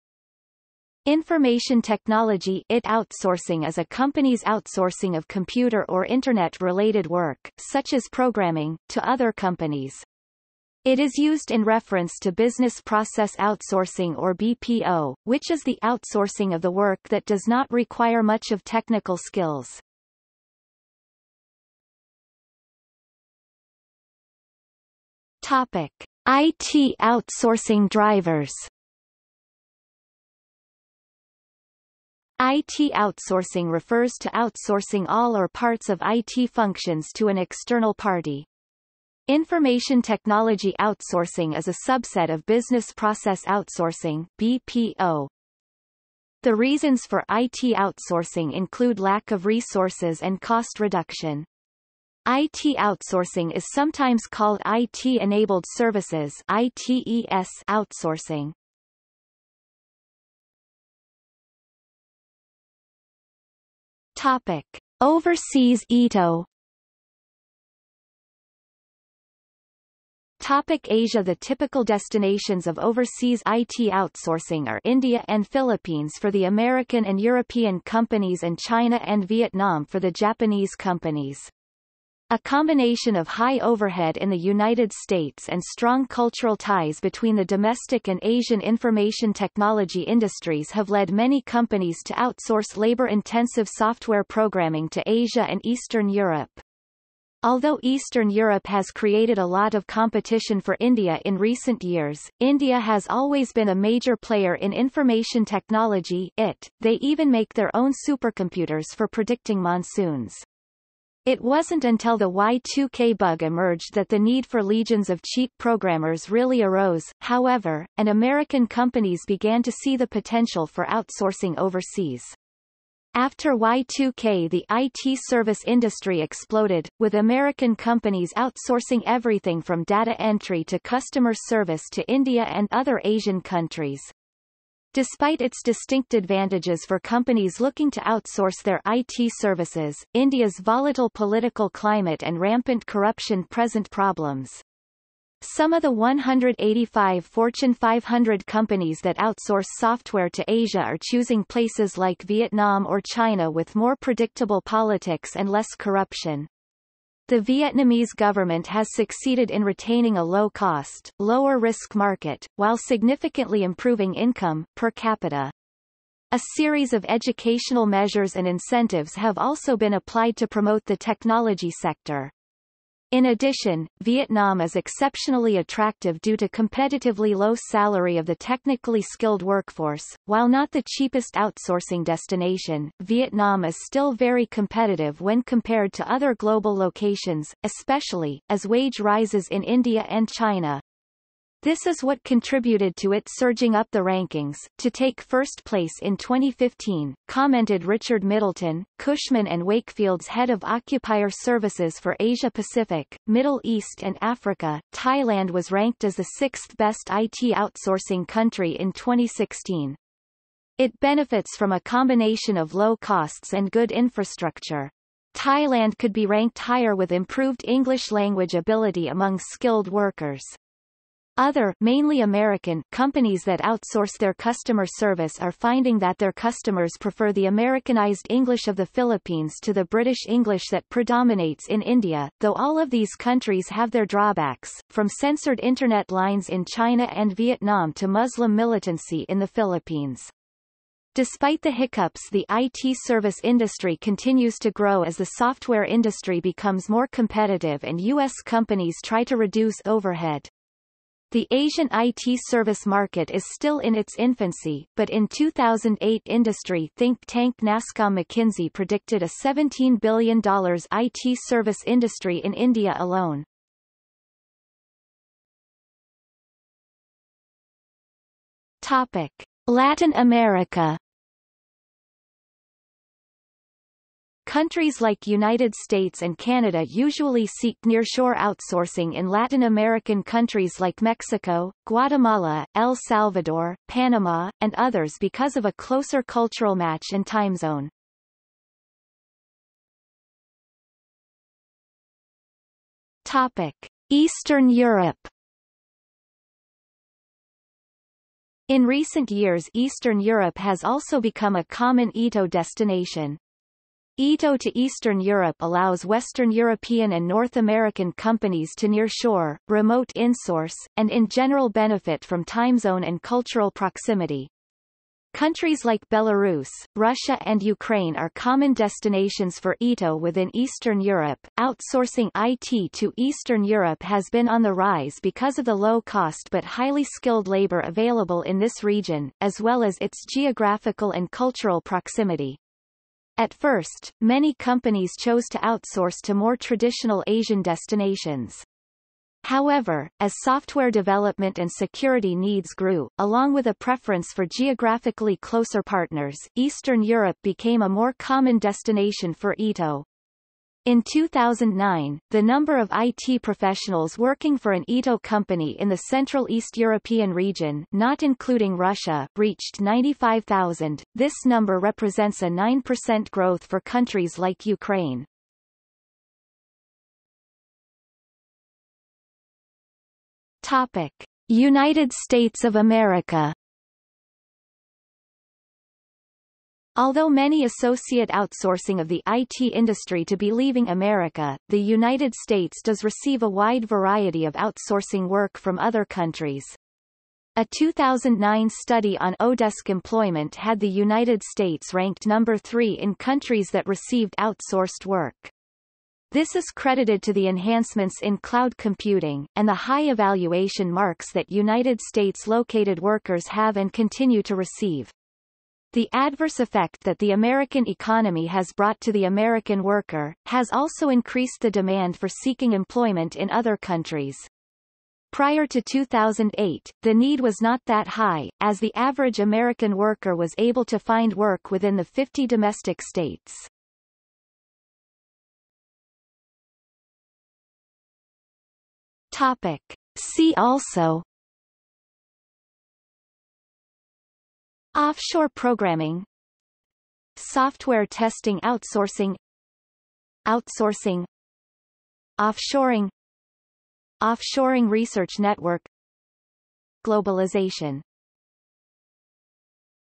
Information technology outsourcing | Wikipedia audio article
00:00:21 1 IT outsourcing drivers
00:01:00 2 Overseas ITO
00:01:10 2.1 Asia
00:04:25 2.2 Latin America
00:04:49 2.3 Eastern Europe
00:06:07 2.4 United States of America
00:07:05 3 See also

- Socrates

SUMMARY
Information technology (ΙΤ) outsourcing is a company's outsourcing of computer or Internet related work, such as programming, to other companies.  It is used in reference to business process outsourcing or BPO, which is the outsourcing of the work that does not require much of technical skills.